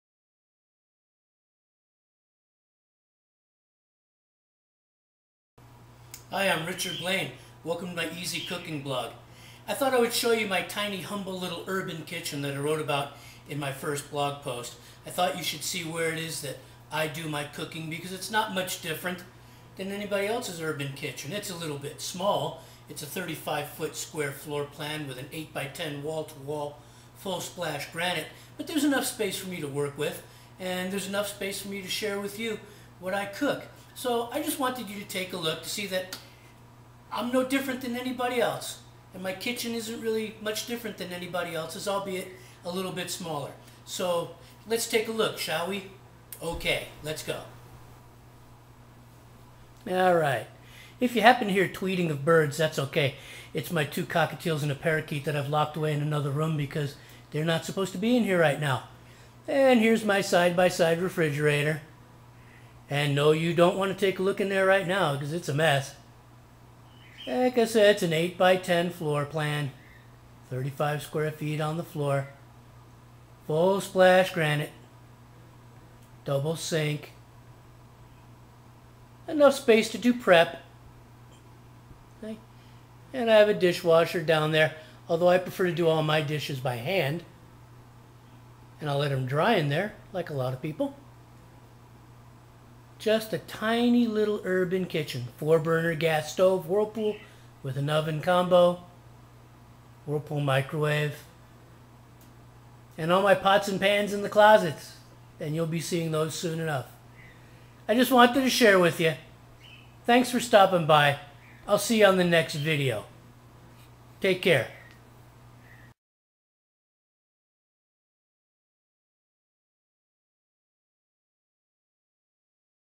Richard Blaine's EZ Cooking Kitchen Intro
This is just an intro to my home kitchen that I will use to share with you!

The Gear I Use To Sharpen My Knives:
DMT W6X
DMT W6F
DMT W6E

DMT W8CXNB
DMT W8EFNB

SPECIAL PLATES:

DMT D8EE

Spyderco 302UF
Spyderco 302U
Surgical Black Arkansas